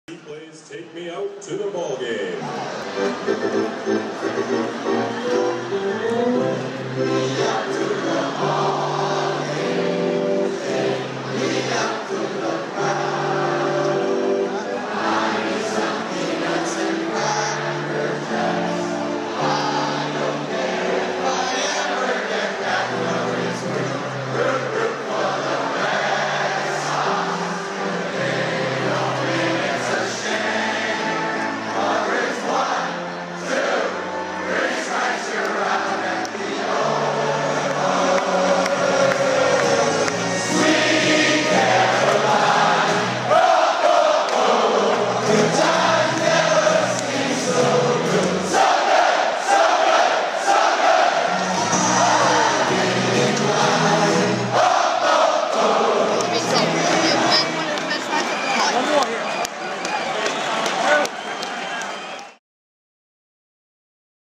A 10-year-old's eye on the World Series win!
Ten-year-old Gavin Spencer documents his trip to game 6 of World Series and the Boston Red Sox's win.